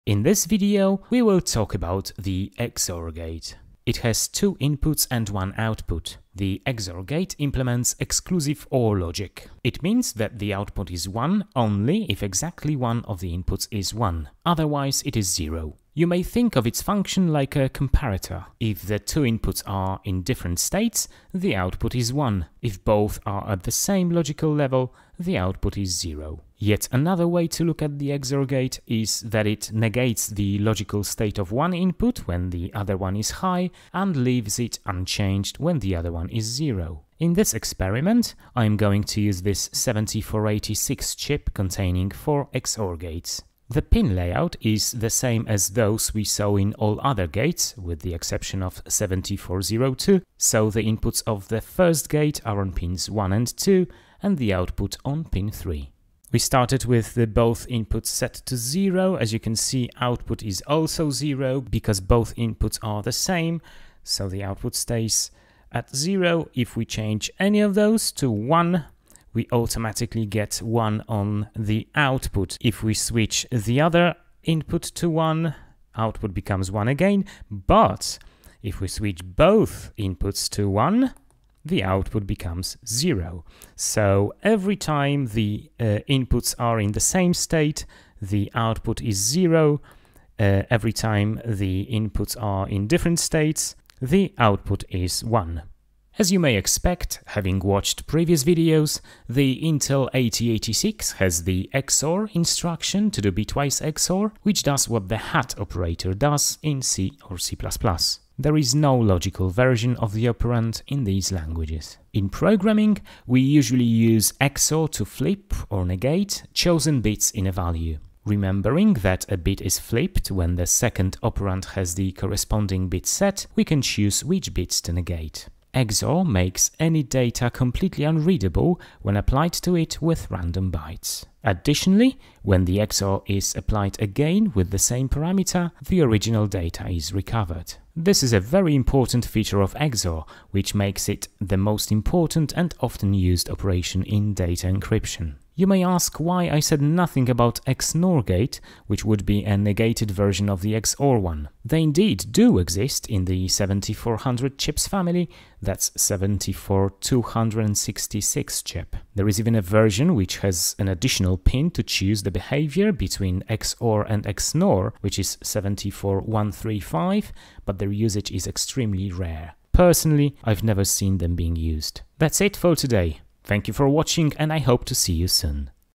The XOR gate - Digital electronics basics: 7400 logic chips [part 5]
Introduction to digital electronics in a tutorial style series of videos aimed mostly at beginners.
This video shows how the XOR gate works using the 7486 chip.